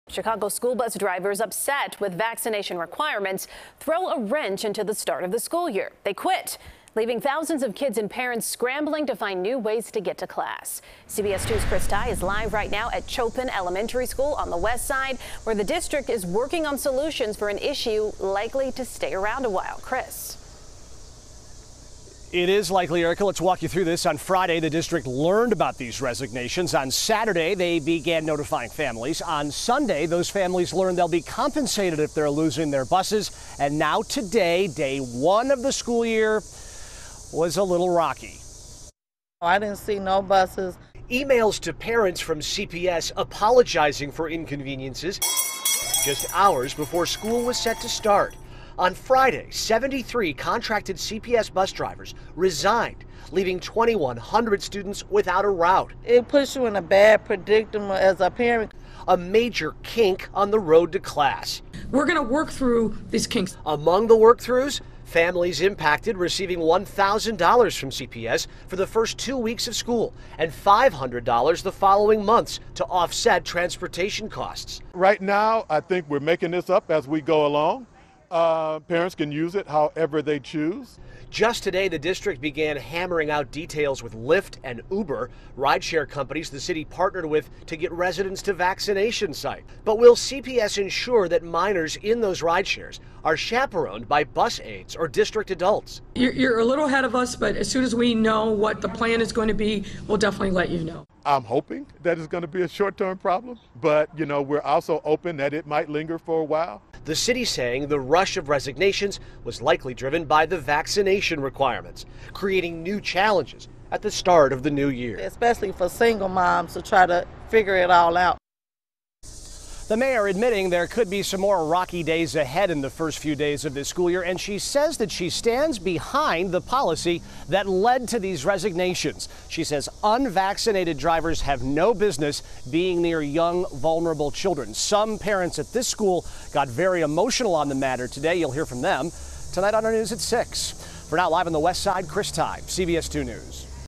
CPS Students Left Without A Route After Dozens Of School Bus Drivers Resigned
This first day of school for the Chicago Public Schools, school bus drivers who are upset with COVID-19 vaccination requirements have thrown a wrench into things. CBS 2's Chris Tye reports.